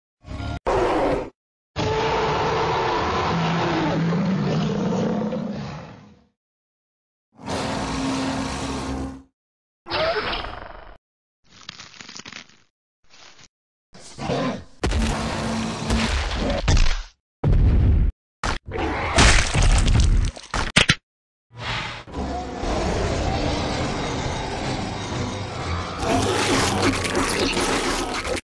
PDW Australovenator vs Ostrich
My 2nd animal that fights a dino! and the ostrich was eating a humongous plant.
Sounded by Para67.
extra sounding by me.
bgs by me and prehi productions
stks: ES1001 for Austra and IDK who made ostrich.
next Fight - Gojirasaurus vs Monolophosaurus